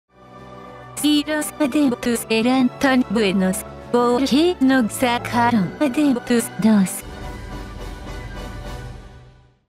Adeptus 2 [Ningguang]
Premio por el concurso semanal del servidor de discord, sugerido por uno de los ganadores: "Eduardo Rivas"
*Este video fue hecho como parodia y con fines de sátira, no genera ingresos y está hecho con el único fin de entretener, no representa nada hecho o distribuido por parte de la compañia MiHoyo.